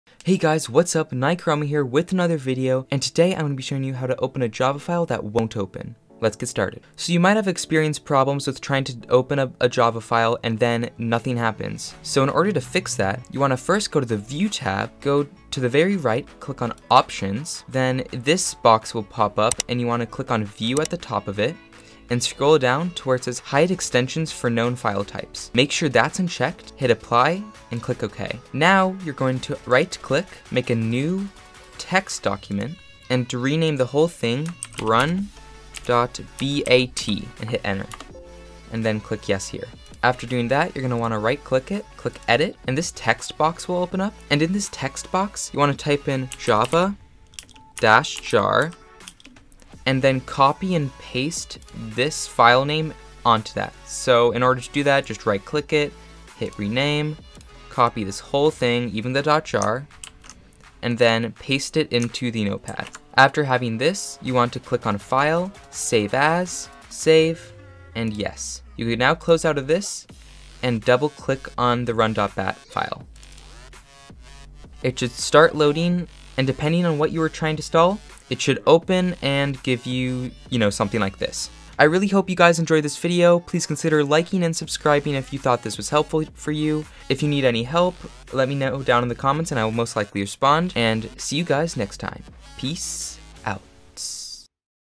How to Open Java Files that Won't Open (Optifine,Shaders,Forge) [Windows 10/11]
Have you tried to open java files that just won't open? Well here is your fix!

Software used to make this video:
Streamlabs OBS
Audacity
Premiere Pro
Photoshop